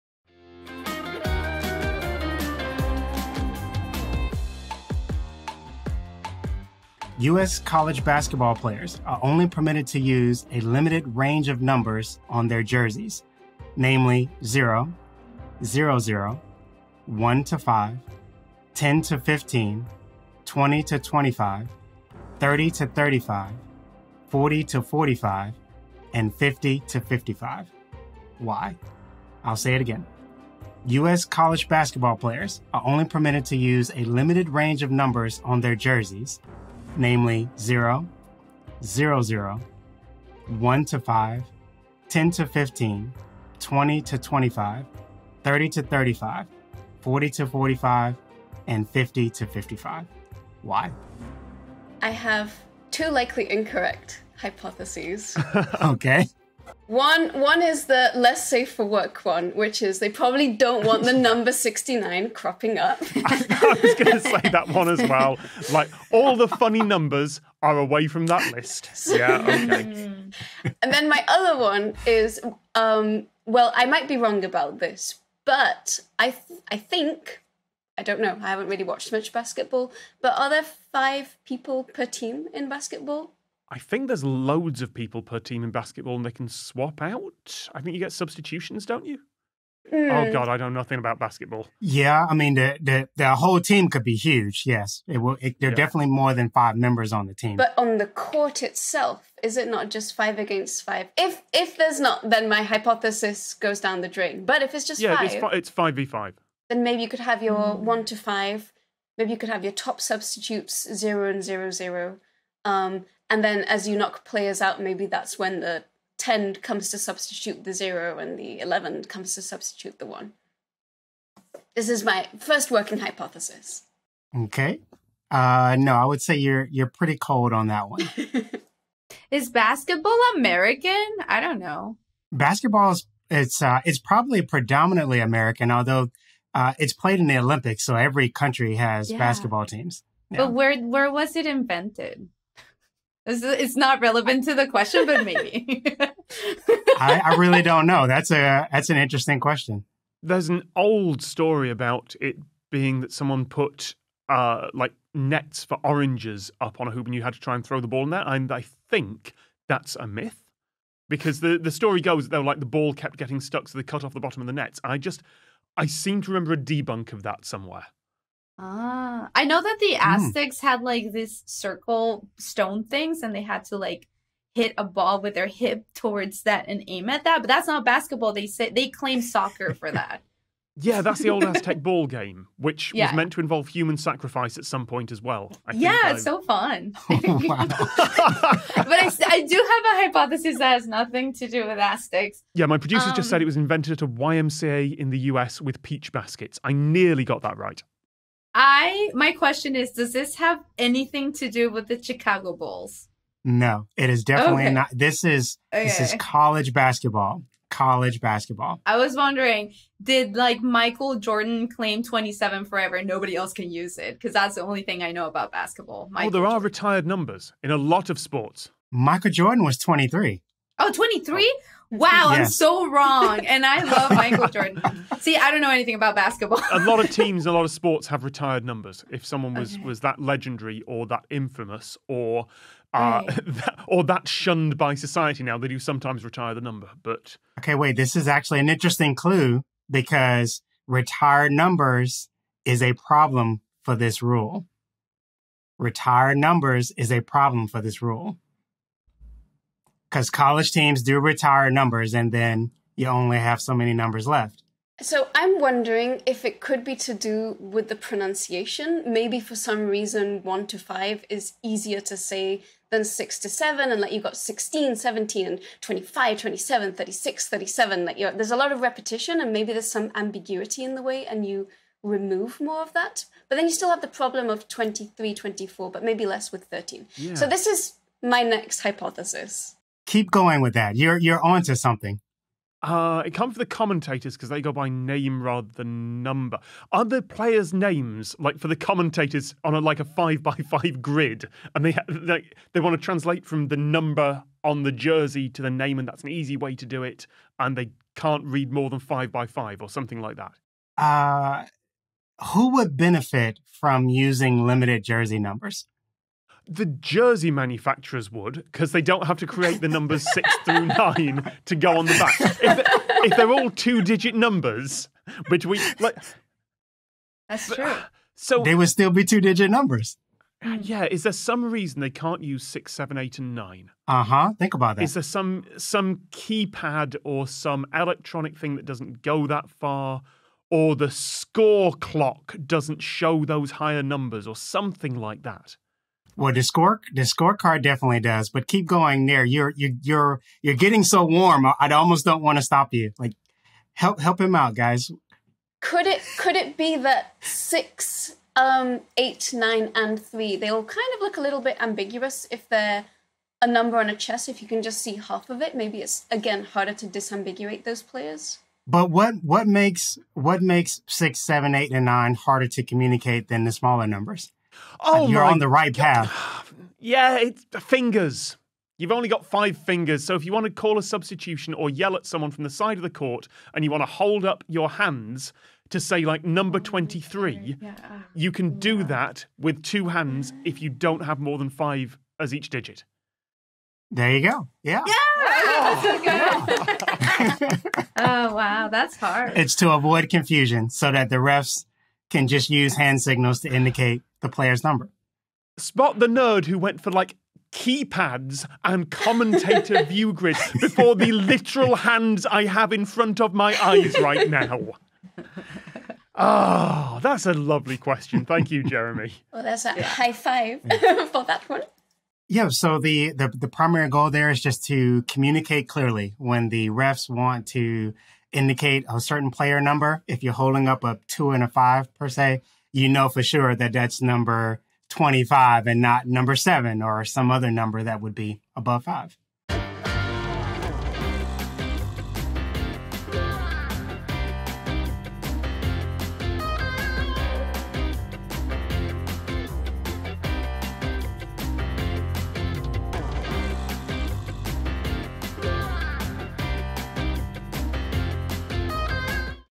The banned numbers in basketball
Jeremy Fielding, Estefannie and Inés Dawson face a question about a sports shirt standard.

QUESTION PRODUCER: David Bodycombe.

RECORDED AT: The Podcast Studios, Dublin.
EDITED BY: Julie Hassett.